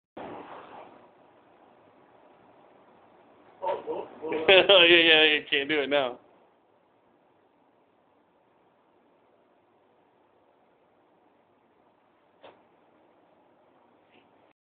Balancing hat
Jeremy Cox balances a base ball hat on his nose like a seal. TOOO COOL!